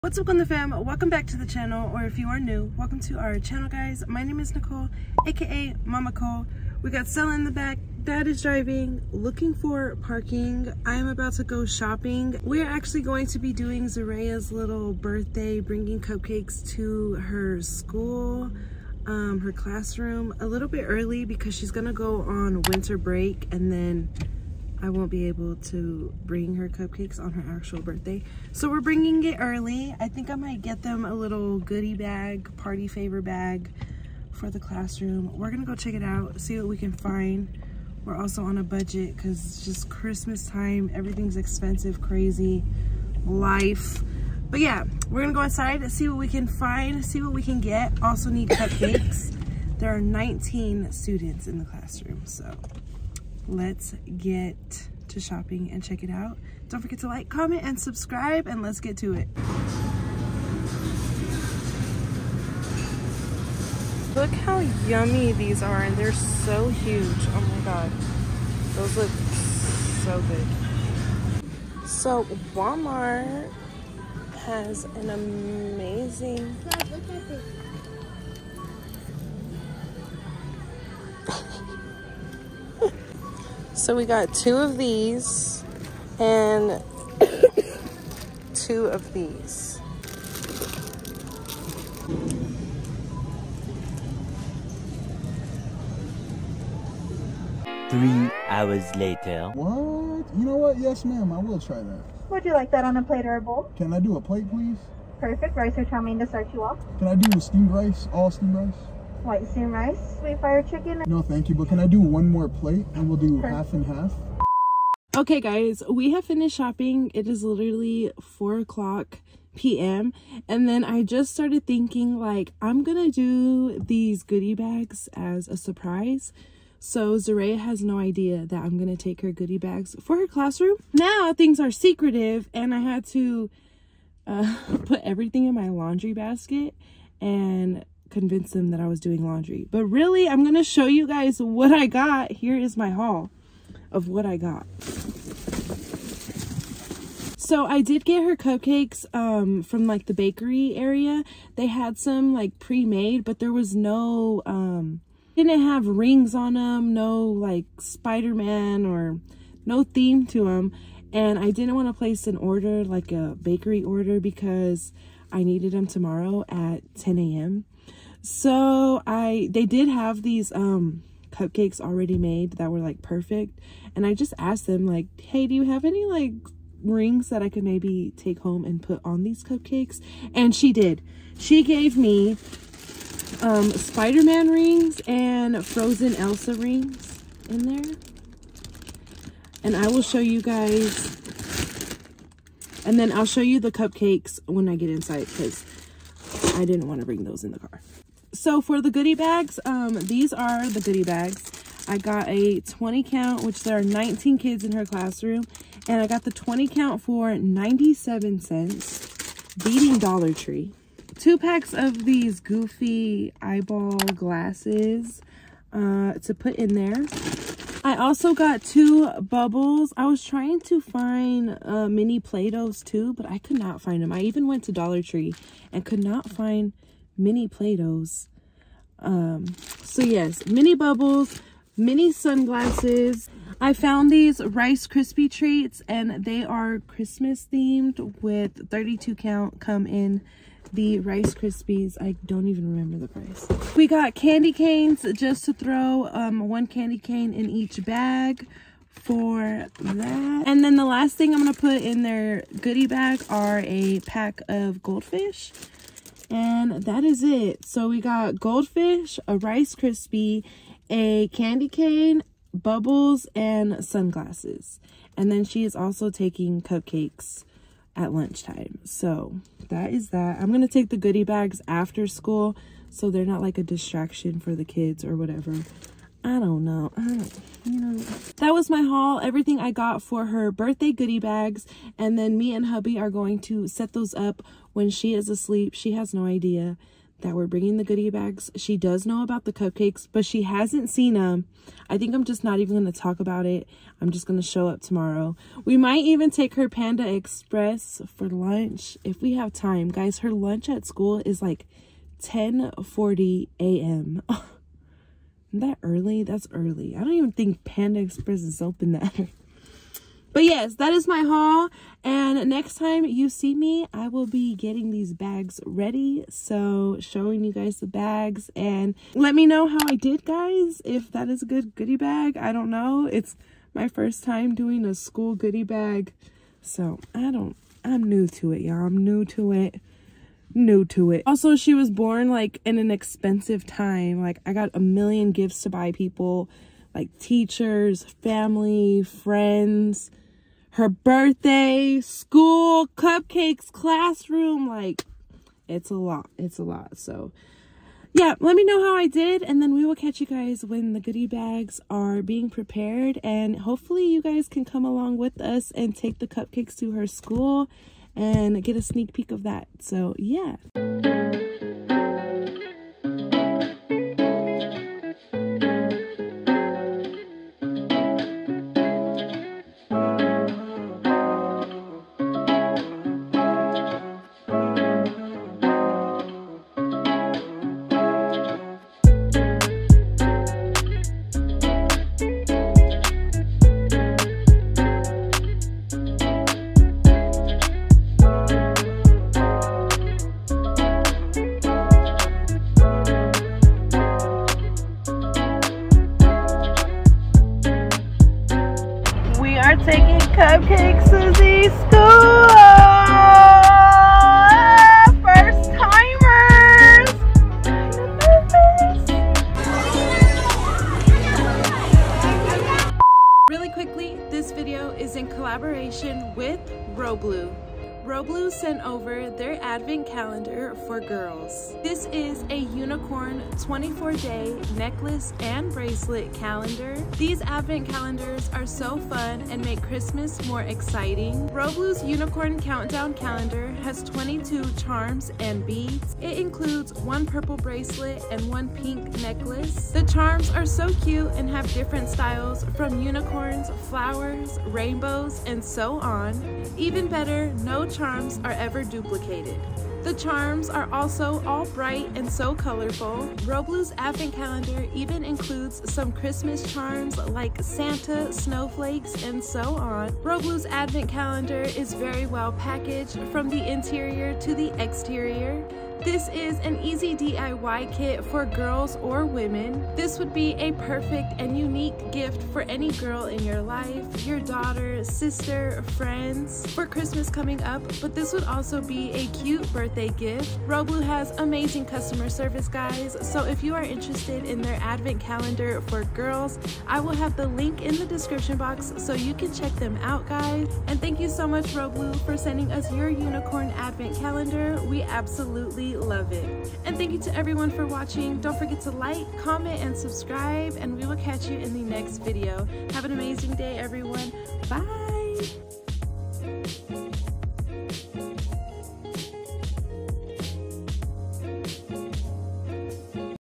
CELEBRATING OUR DAUGHTERS BIRTHDAY AT SCHOOL ♡ WALMART HAUL ♡ ROBLUE ADVENT CALENDAR
We are Living Conde and welcome to our family ♡
Todays vlog we are celebrating our daughters birthday at school! This video is in collaboration with Roblue. Have an amazing day everyone!

ROBLUE NECKLACE ADVENT CALENDAR: